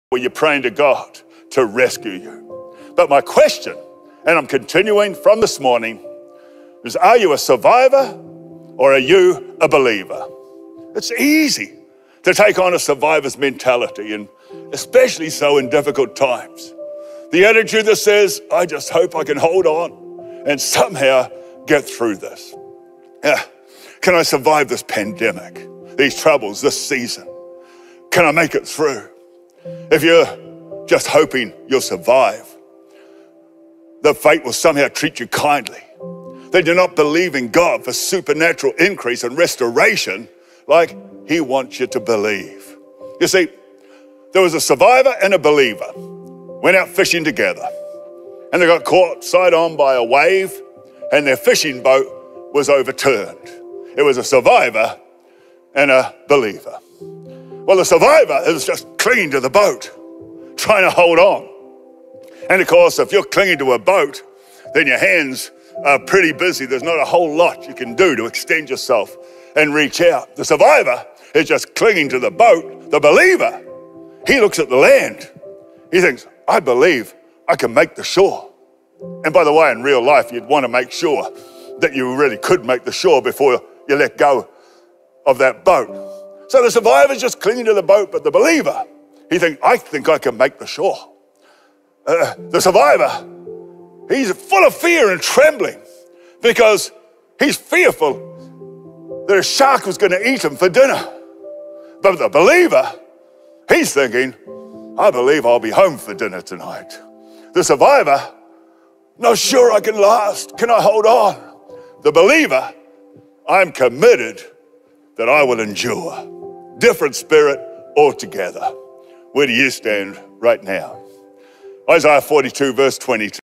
PASTOR BRIAN GASLIGHTS AND MOCKS SURVIVORS 2021 OCT 3
UNBELIEVABLE gaslighting from Brian Houston who has never dealt with the idea of addressing survivors or victims